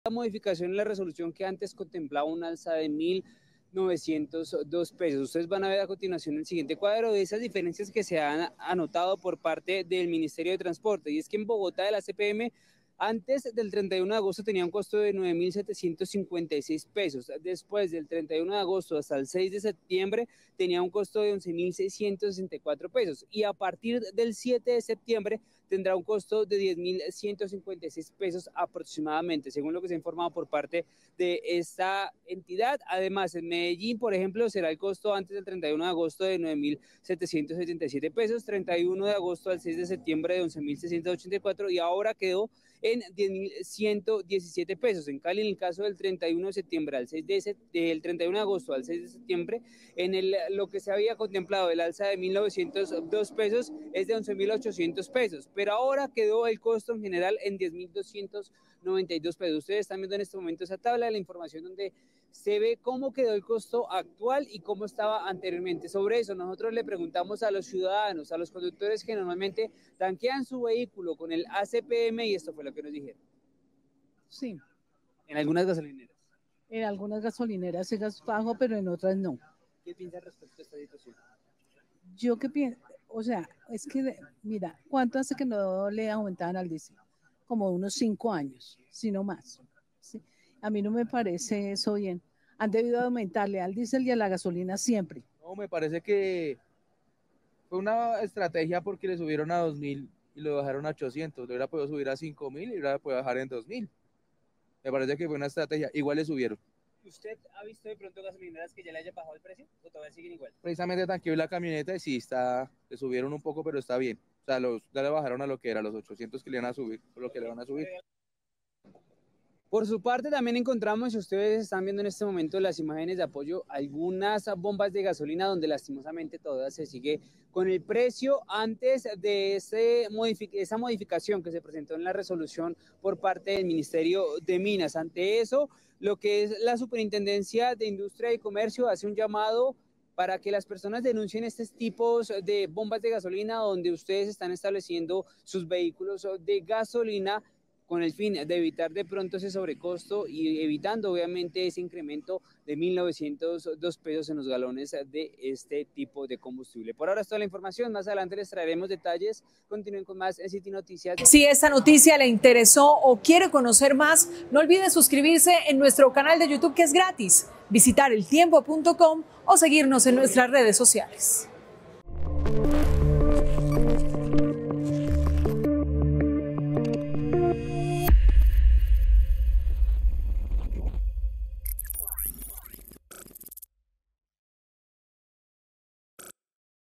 Estaciones en Bogotá estarían cobrando tarifa del Diésel que fue derogada | El Tiempo
Pese a que desde el sábado rige el primer incremento de 400 pesos, algunas estaciones aún registran el precio del combustible con el alza de 1.904 pesos, que finalmente fue derogado. En la capital, el precio debe quedar en 10.116 pesos aproximadamente.